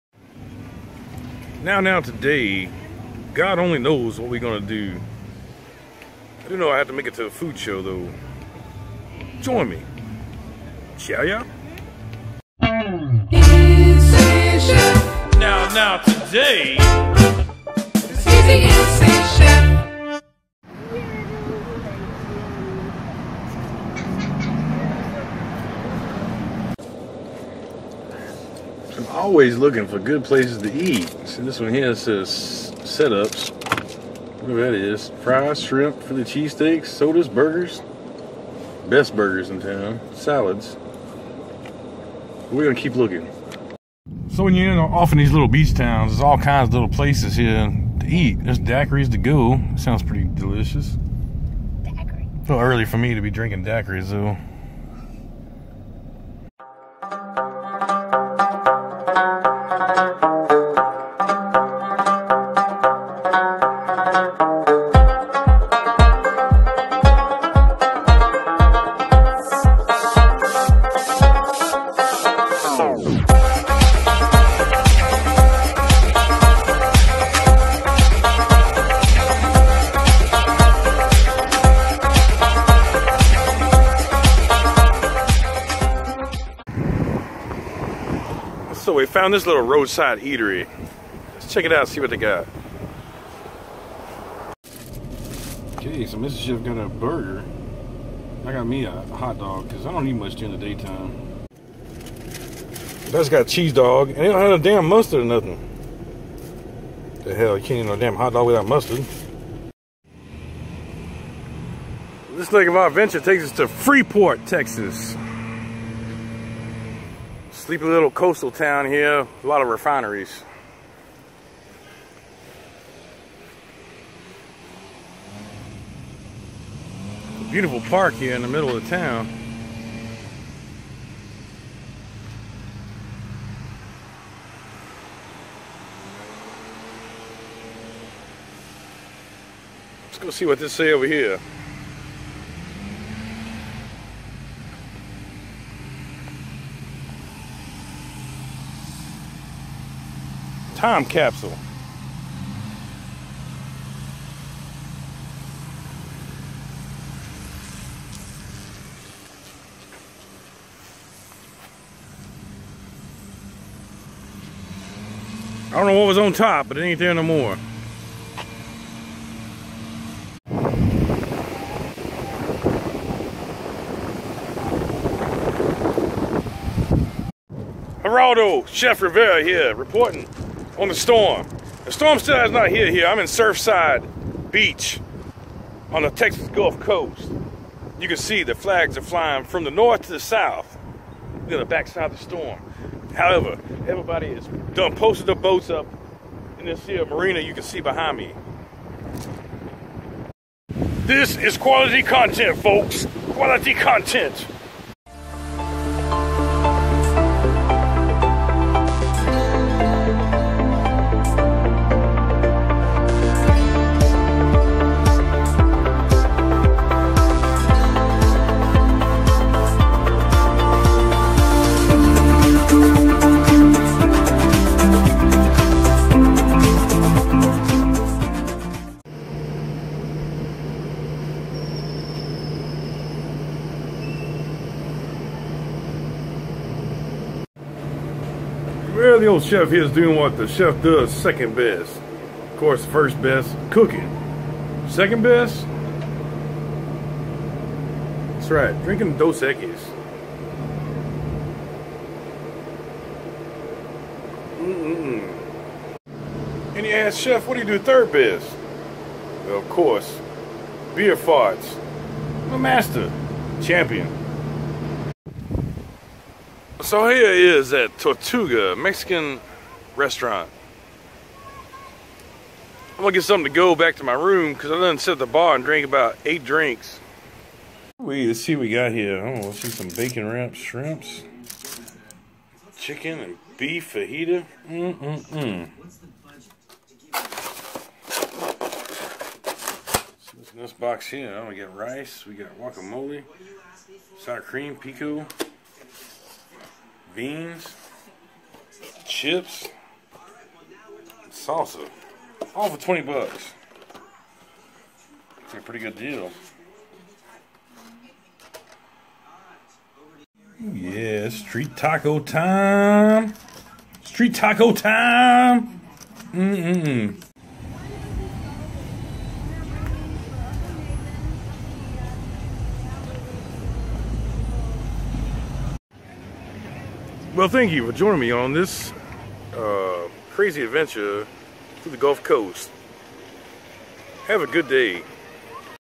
InSane on The Road Texas Gulf Coast
Today we check out some coastal towns, food, and check in with Geraldo Chef Rivera for a weather report